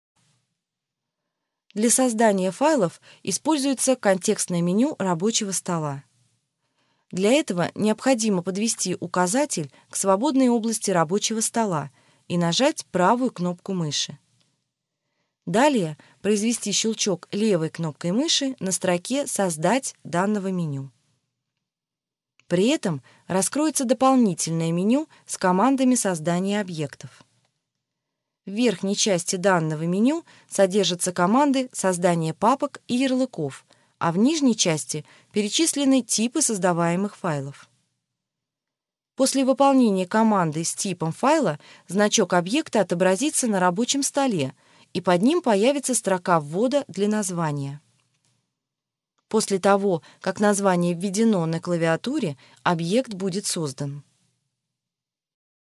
Создание файлов в Windows XP (19/47)
- еще больше обучающих роликов по операционным системам ответят на ваши вопросы на нашем сайте бесплатно!

 Для создания файлов используются контекстное меню Рабочего стола. Для этого необходимо подвести указатель к свободной области рабочего стола и нажать правую кнопку мыши. Далее произвести щелчок левой кнопкой мыши на строке «Создать» данного меню. При этом раскроется дополнительное меню с командами создания объектов. В верхней части данного меню содержаться команды создания папок и ярлыков, а в нижней части перечислены типы создаваемых файлов. После выполнения команды с типом файла значок объекта отобразится  на Рабочем столе и под ним появится строка ввода для названия. После того как название введено на клавиатуре, объект будет создан.